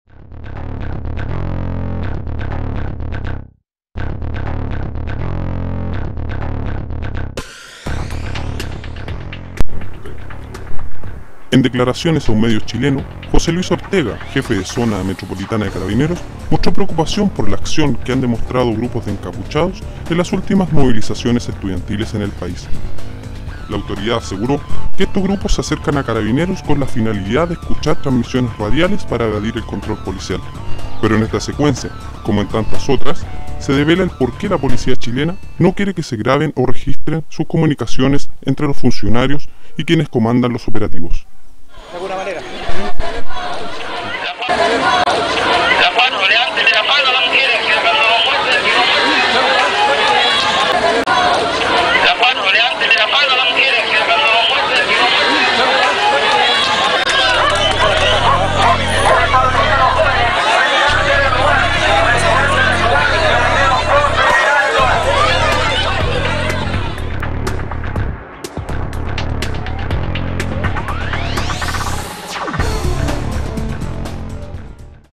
Carabinero FFEE ordena subirle la falda a escolares.-Octubre 2011-
Chile: En declaraciones a un  medio de comunicación chileno,  José Luis Ortega, jefe de zona metropolitana de Carabineros, mostró preocupación por la acción que han demostrado grupos de encapuchados, en las las últimas movilizaciones estudiantiles en el país. La autoridad aseguró que estos grupos se acercan a Carabineros, con la finalidad de escuchar transmisiones radiales, para evadir el control policial. Pero en esta secuencia como en tantas otras, se devela el porqué la policía chilena, no quiere que se graben, ni registren sus comunicaciones, entre funcionarios y quienes comandan los operativos.